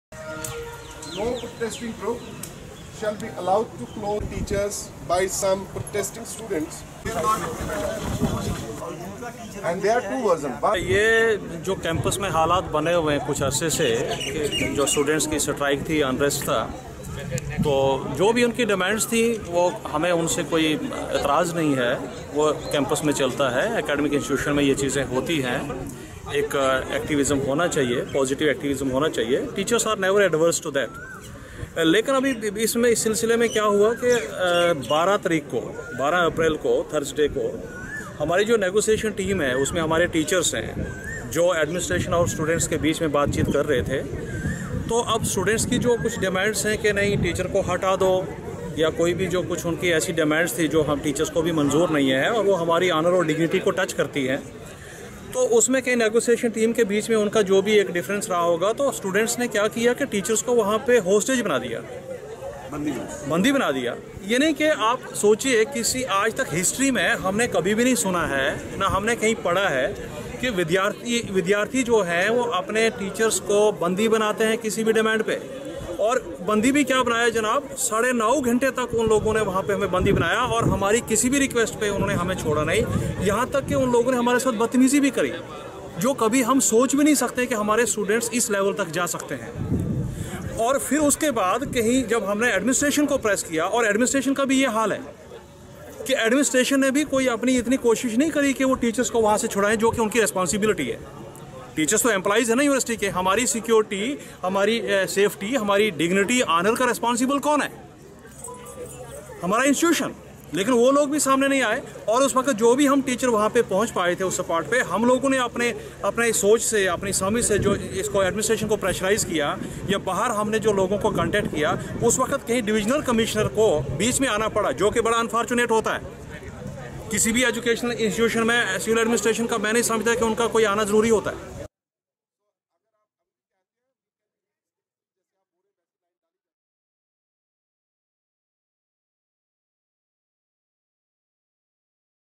JU teachers protest against misbehavior by students
| Excelsior News |